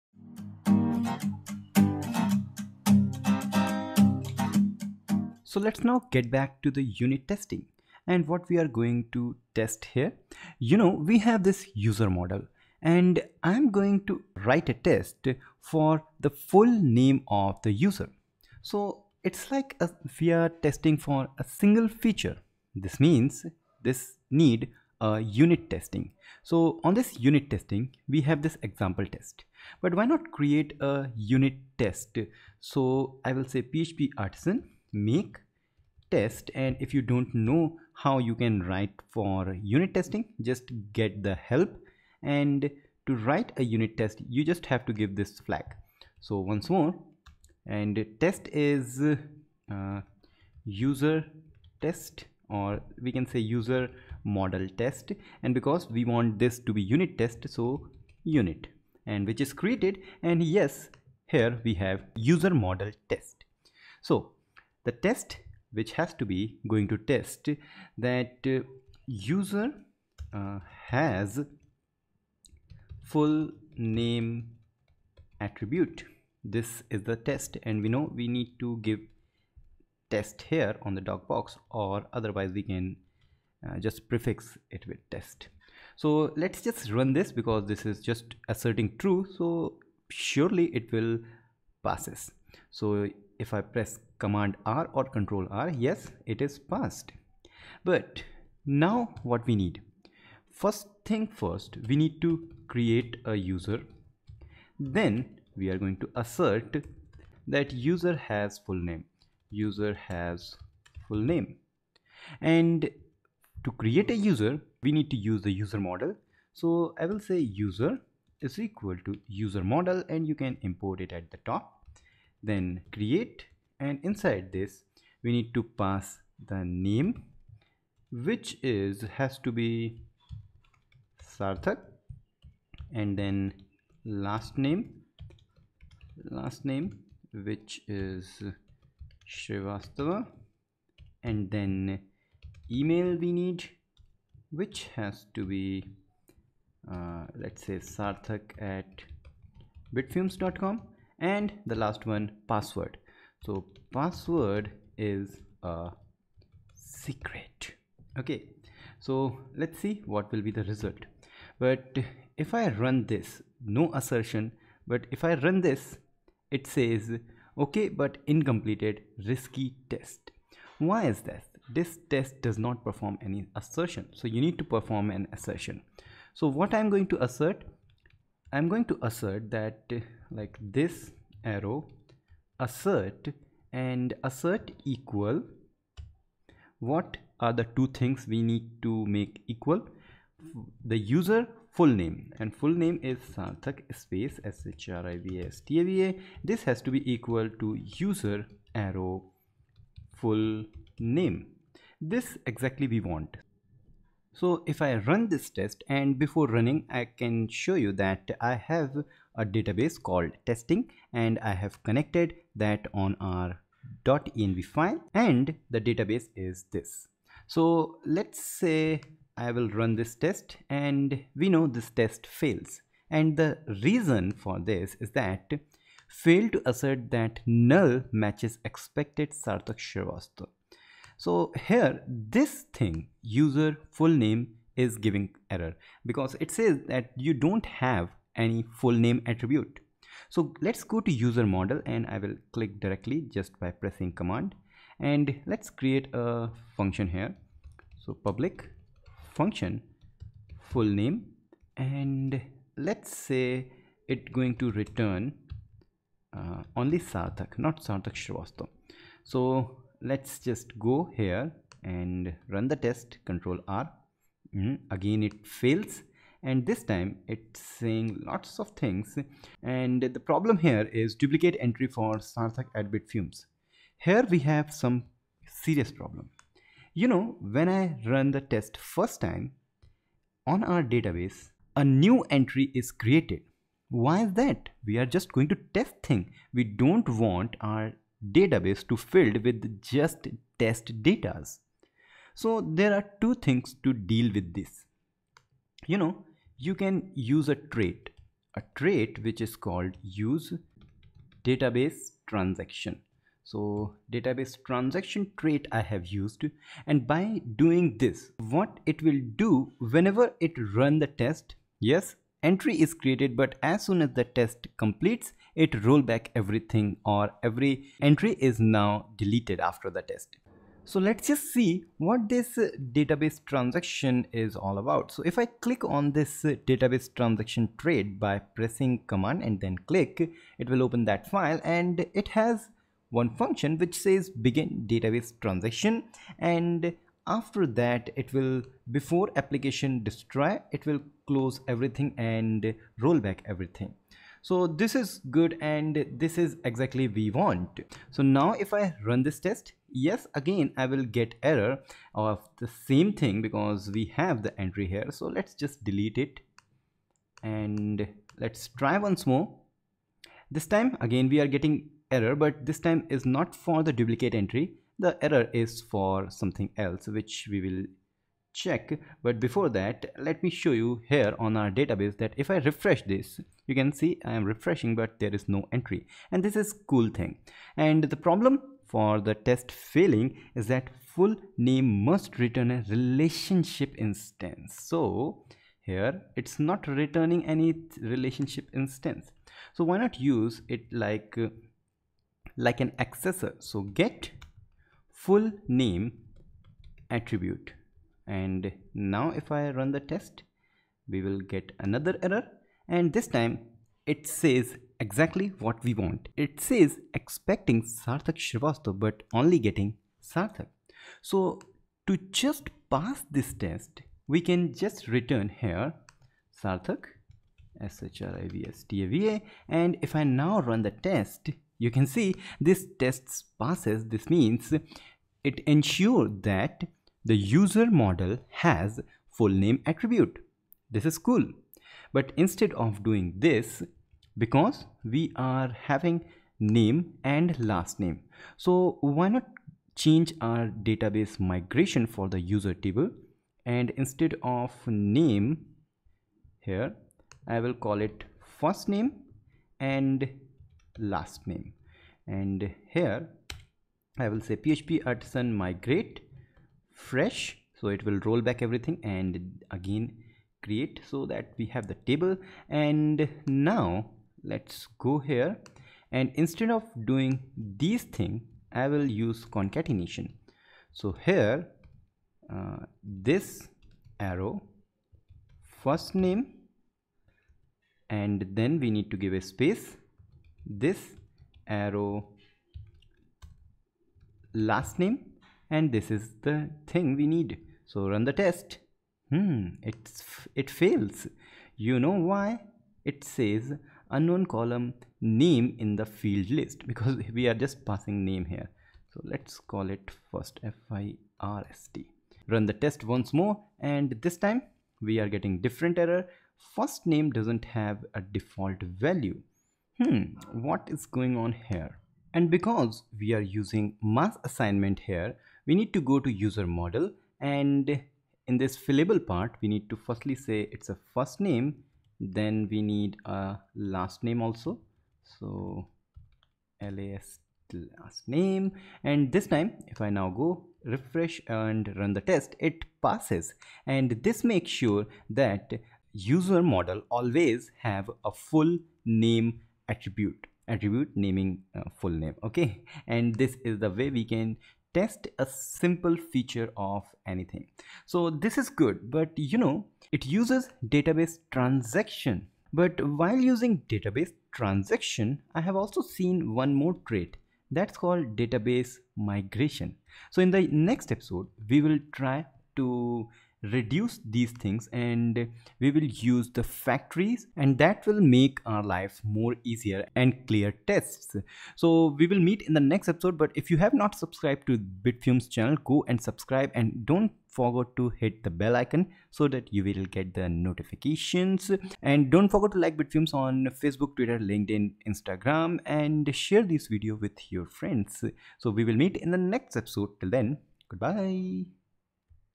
What is Database Transaction in Laravel Testing | Code Testing #6
Testing a Model with Unit Test, What is DatabaseTransaction

FOLLOW ME

--- QUESTIONS? ---